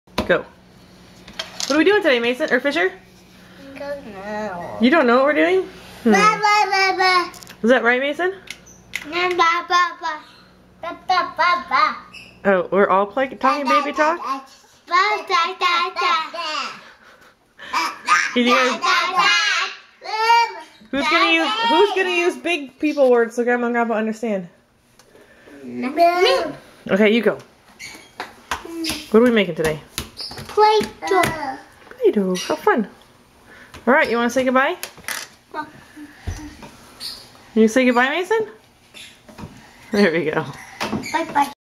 Green Playdoh to pass the day
Nothing like green playdoh to keep a few kids busy for a couple hours!